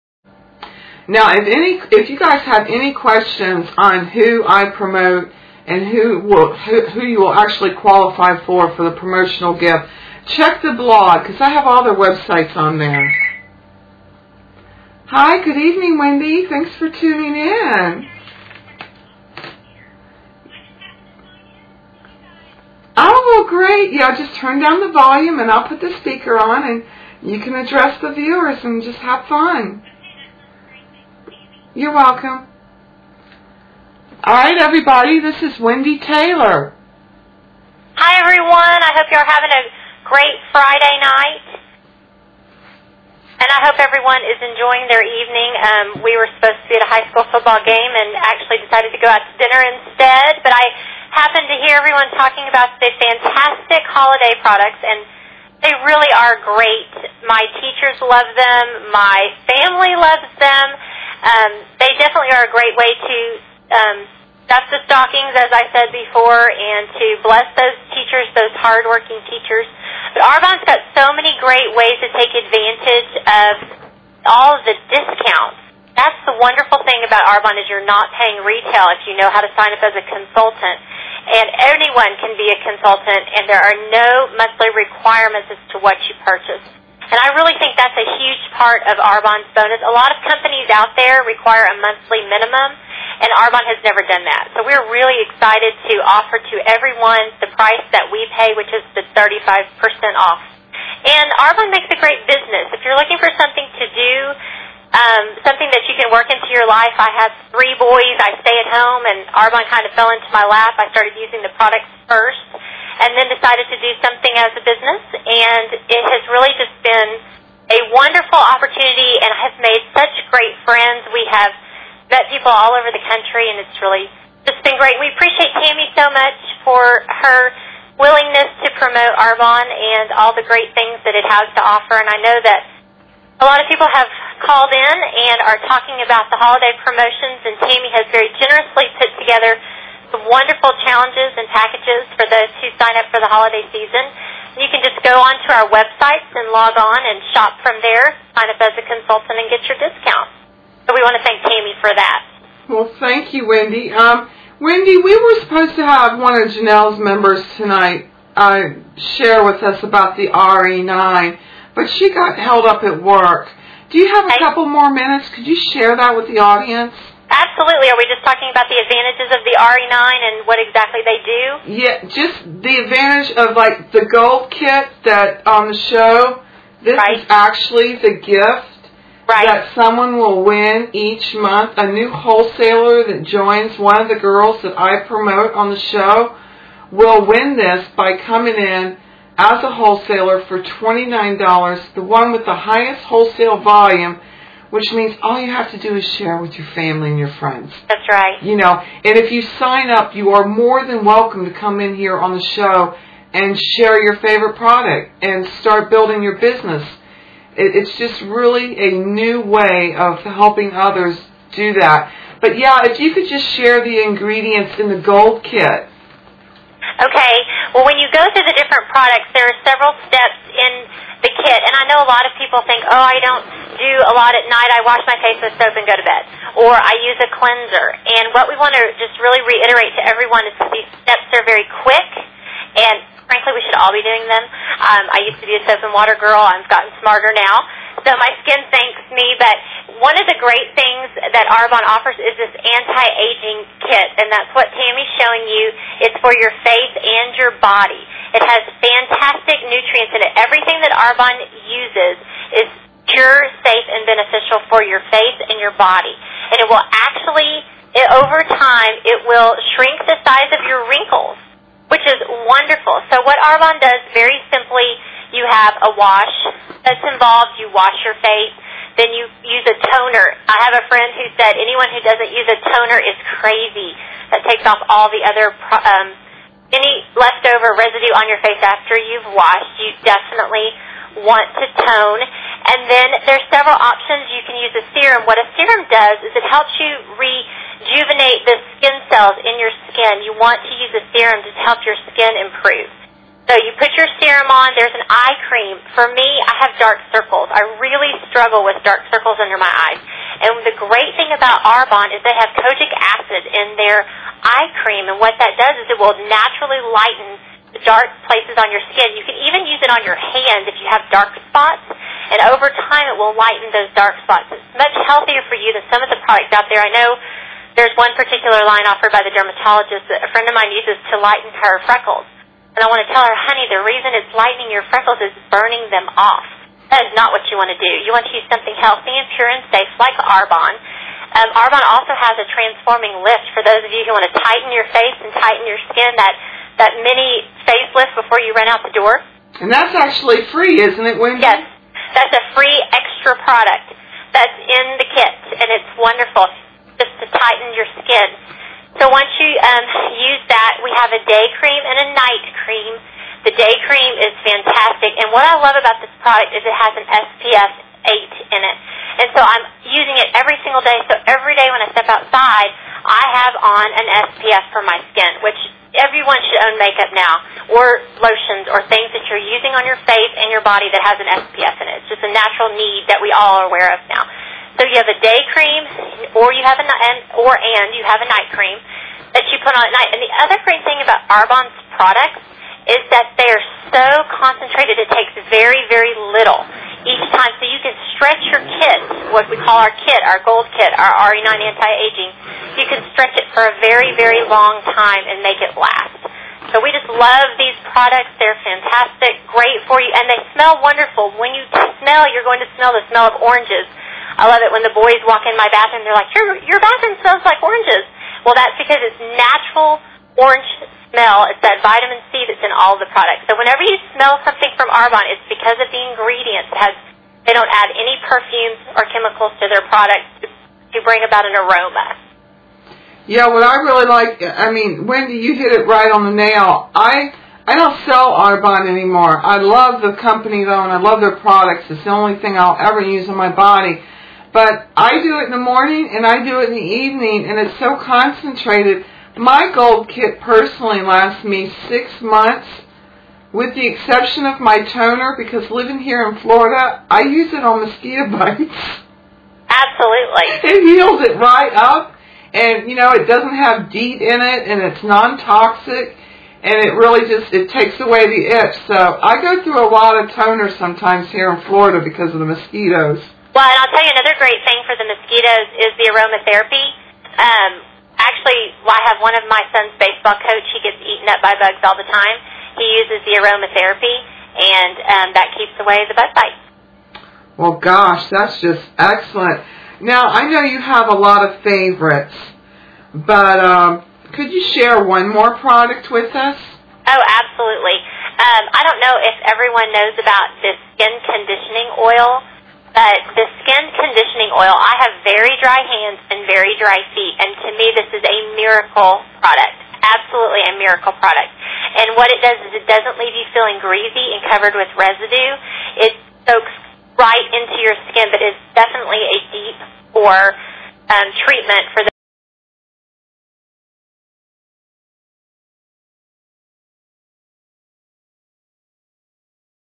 Wendy Taylor on 10/10/08 Holiday Promotional Challenge for Arbonne Team
Wendy Taylor on Holiday Promotional Challenge for Arbonne Teams shares the benefits of anti-aging make-up, pure, safe, beneficial swiss skin care products from Arbonne.  I recommend visiting her website and signing guest book Thank you, Tammy Dantzler